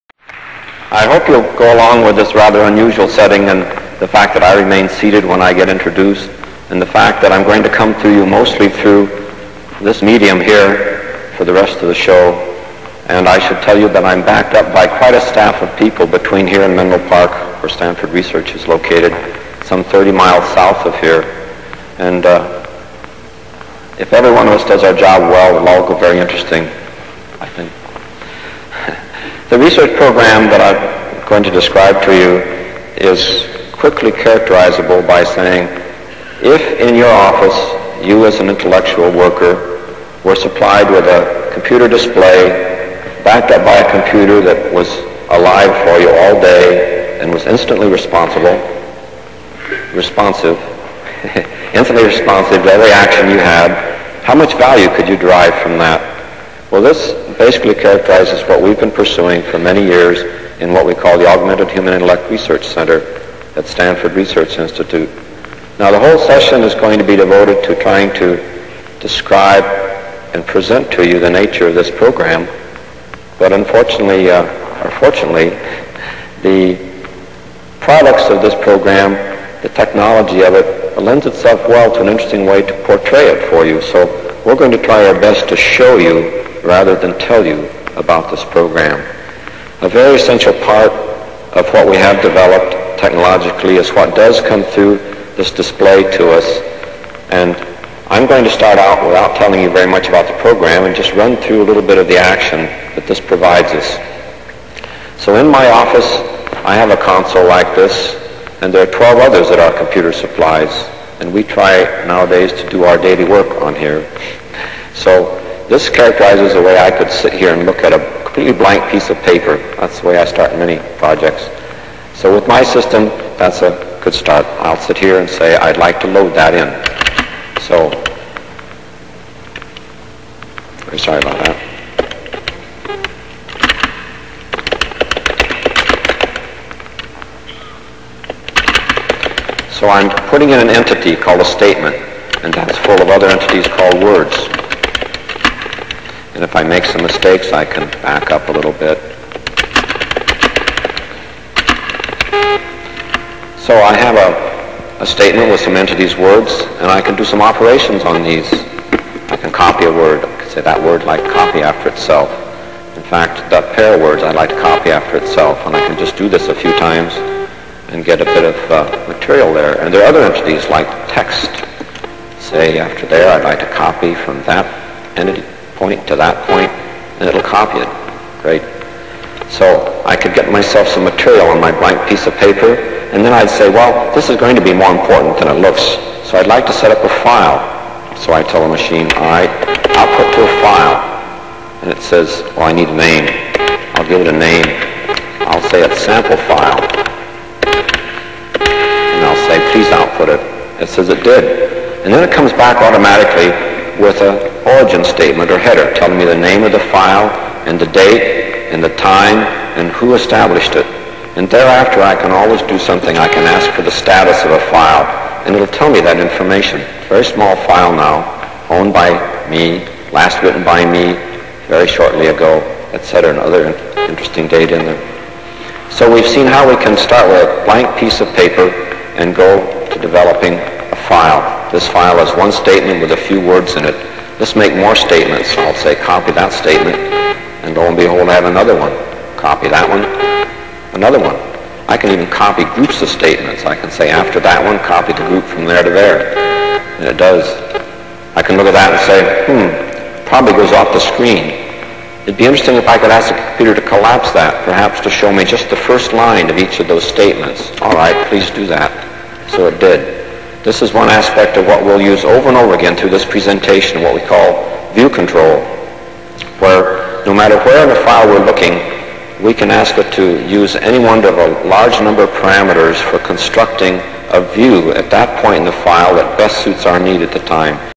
SRI's "Mother of All Demos", Dec. 9, 1968: Introduction
Introduction of the On-Line System (NLS) and word processing (the first popular text editor, EMACS, was still almost a decade away).